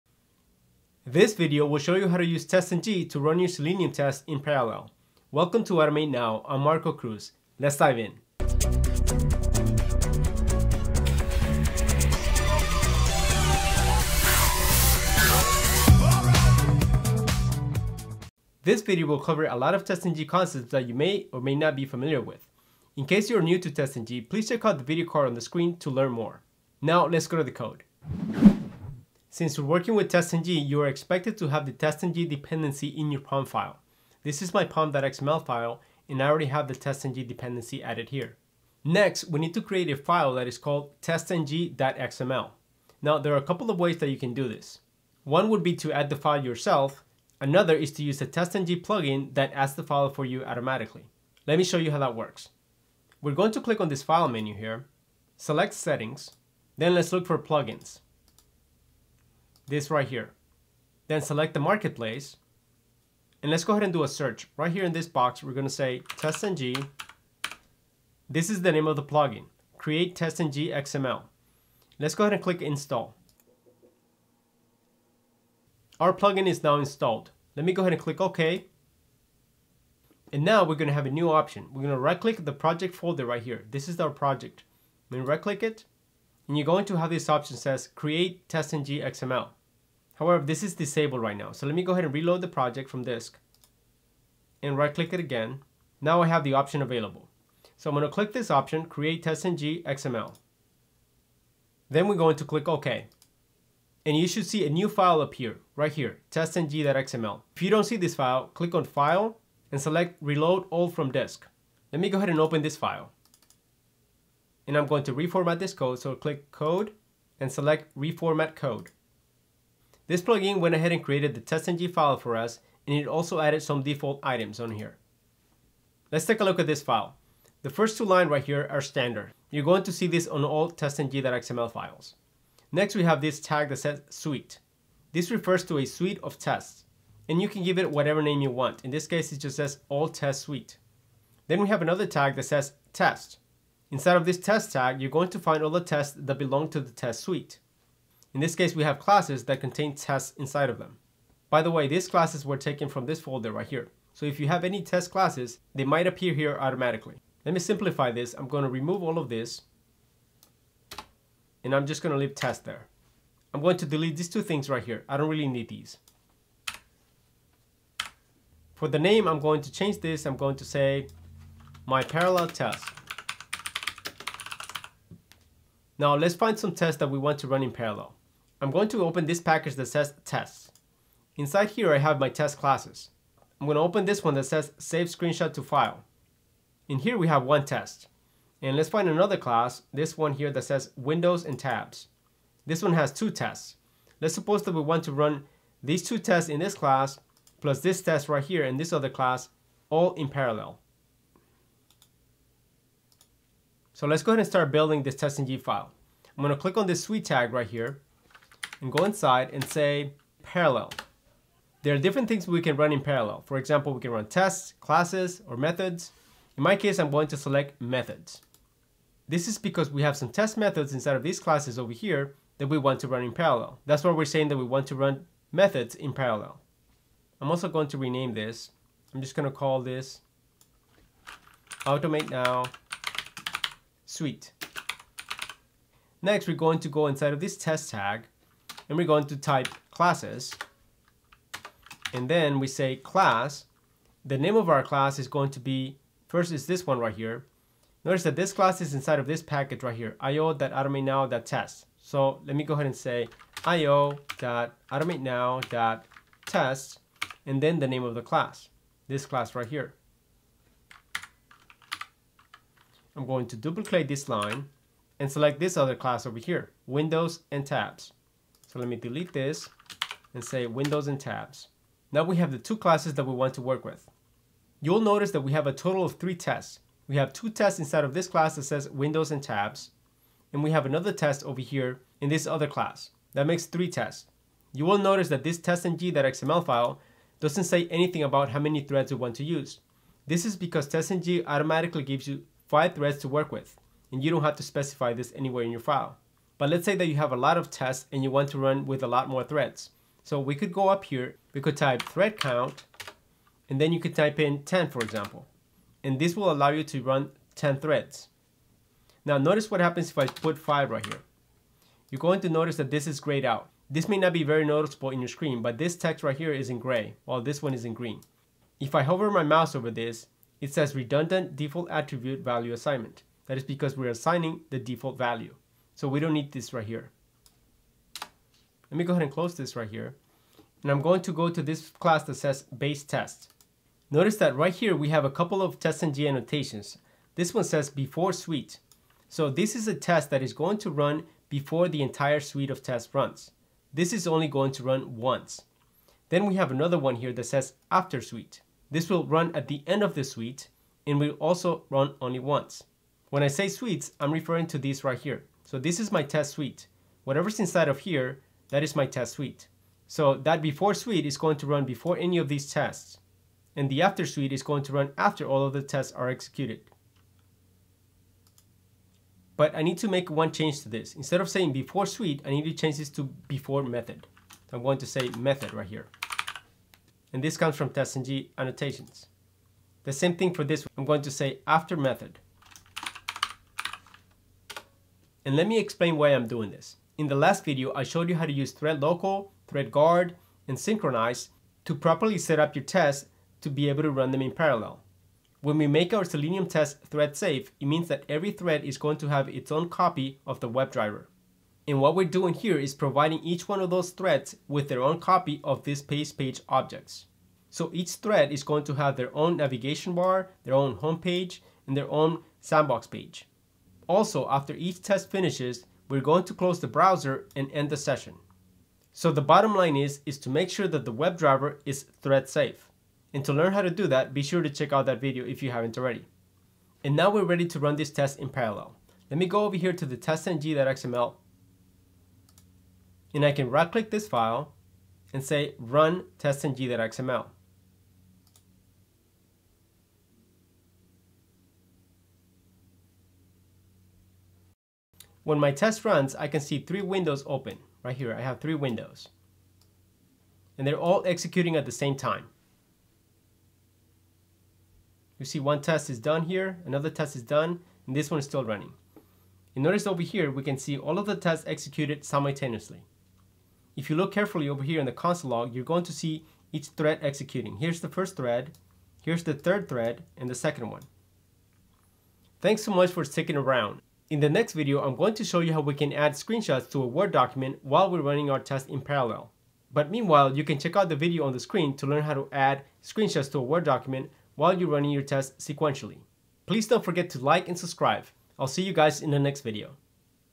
Run tests in parallel in Selenium [EASIEST WAY] | TestNG | automateNow
Learn how to run tests in parallel in Selenium with TestNG.

This video tutorial will show you how easy it is to run your Selenium WebDriver tests in parallel using one of the most popular testing libraries--TestNG. You will learn about the significance of the testng.xml file and how it can be used to run tests, classes, or methods in parallel.

👨‍💻Learn Selenium like a pro with our highly-rated courses!

📑 CHAPTERS
0:00 ~ Intro
0:29 ~ How To Add testng.xml File
1:59 ~ What To Put In testng.xml File
4:33 ~ Running With Multiple Threads
5:30 ~ TestNG Before Suite & Before Method
7:06 ~ Run Tests In Parallel
7:51 ~ Outro